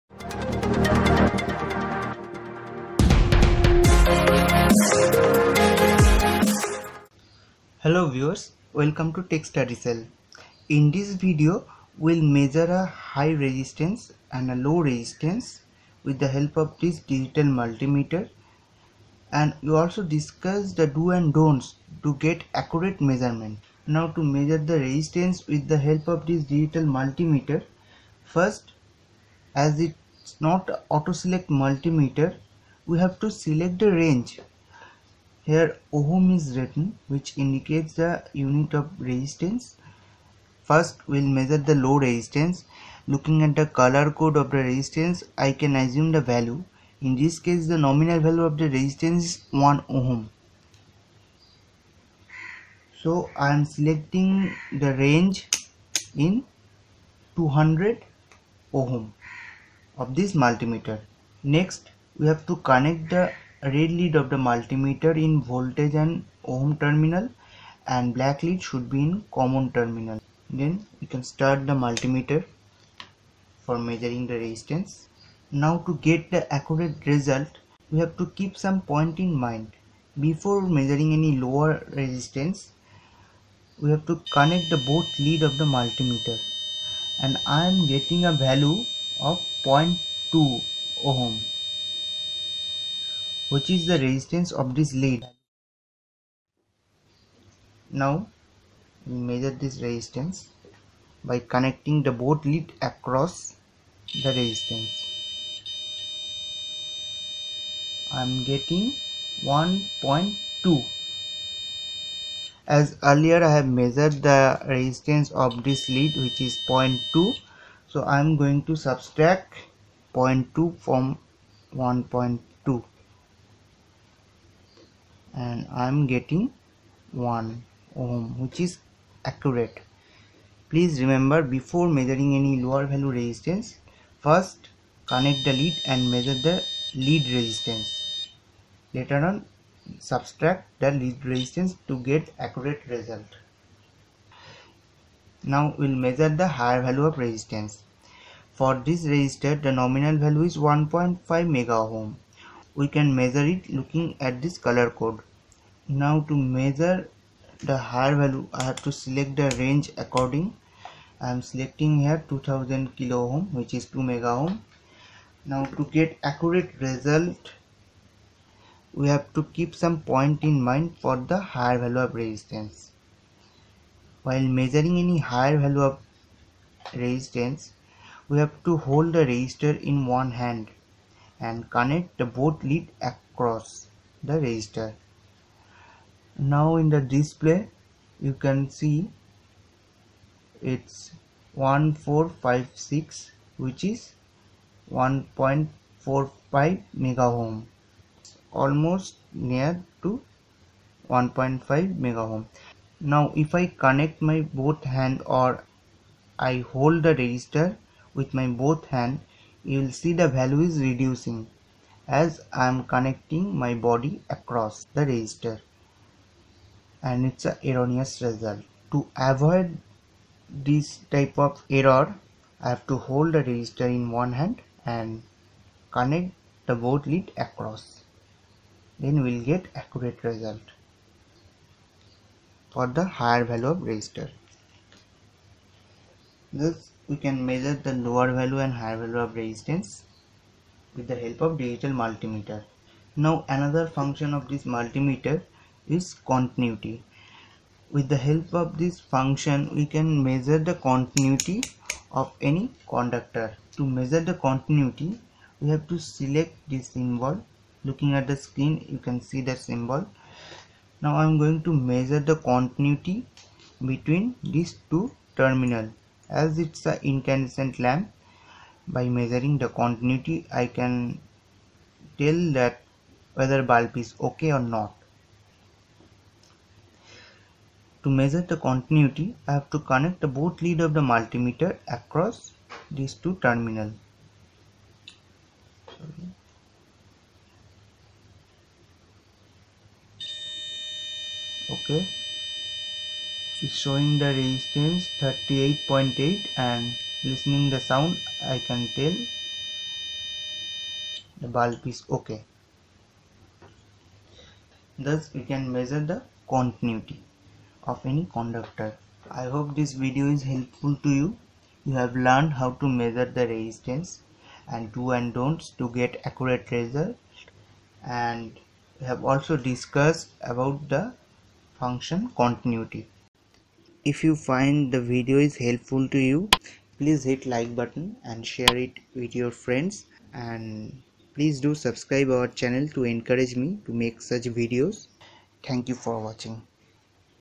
How to measure Resistance with Digital Multimeter / Multimeter tutorial
In this video, I go through some of the basic functions of a Digital Multimeter,
1. How to measure Resistance by using Digital Multimeter.
2. Do and Don't to get accurate measurement for both high & low resistances,
3. How to check continuity by using Digital Multimeter.